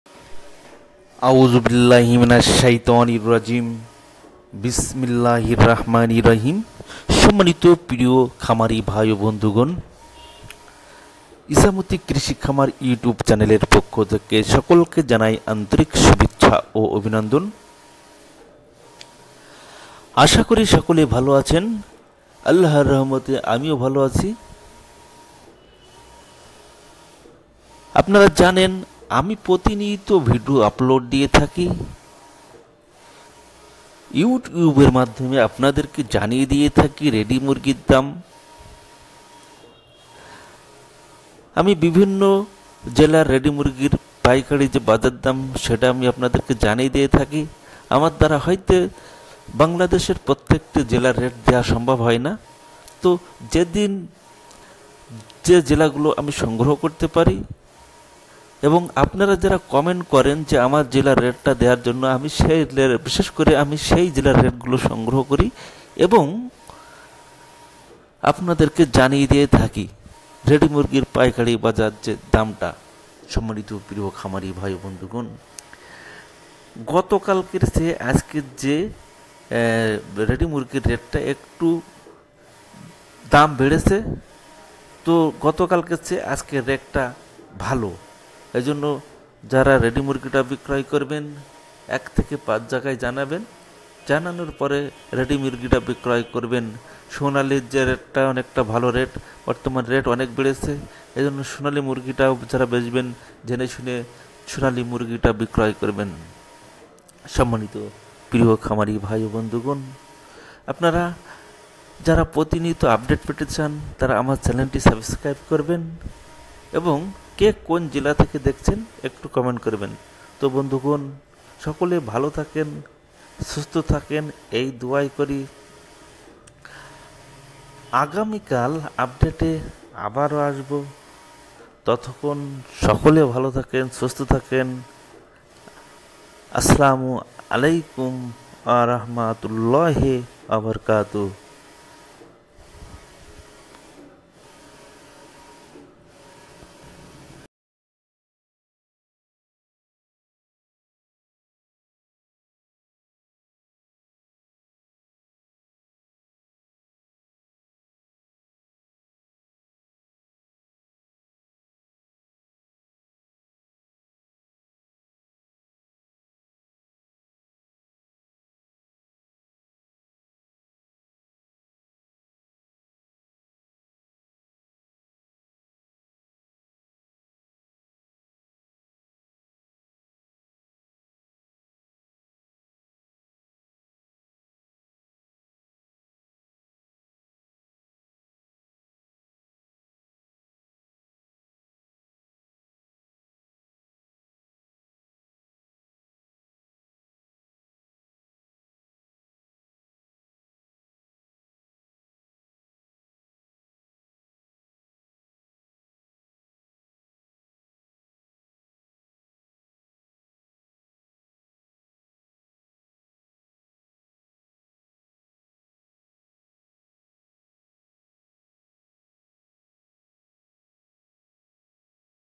২১/৪/২০২২ আজকের ব্রয়লার সুনালি বড়ো রেডি মুরগির পাইকারি বাজার দাম জেনে নিন
# ajkerpaikairi_murgir_bazar_o_ak_diner sokol_has_murgir_baccar_dam_o_paikari_bazar_dam